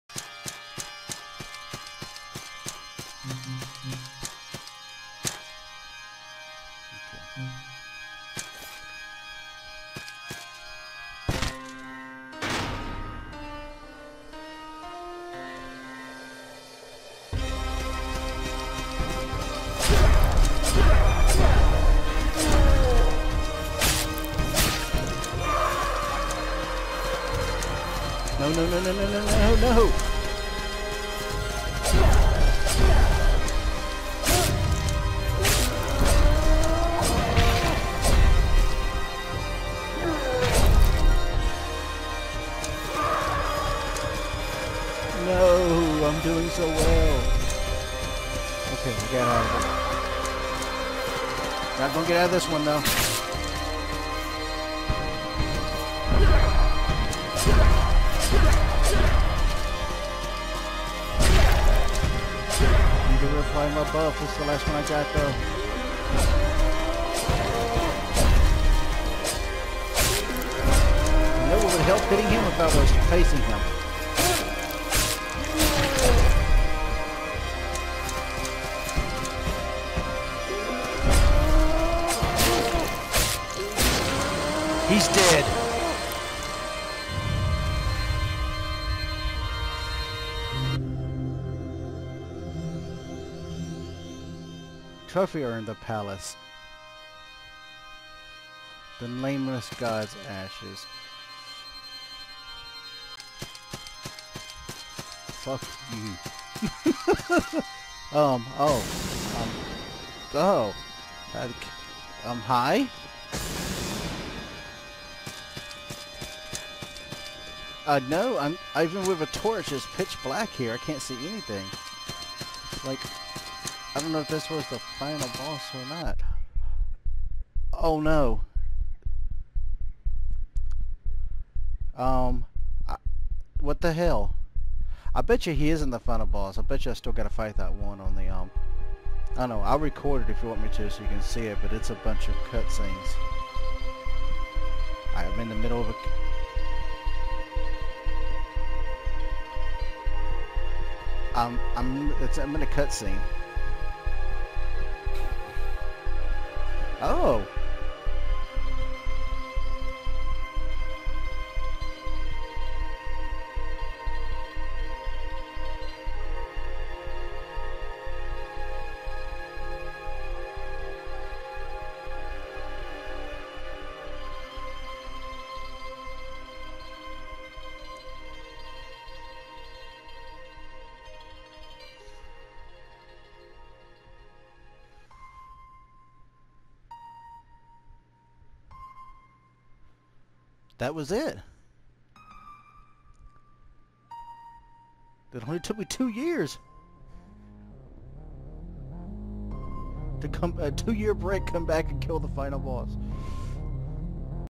Nameless God (End Game) (2 Year Break To Finish on PS4)
I completely forgot about this game honestly. Well, 2 year break is what I needed to beat him it seems lol.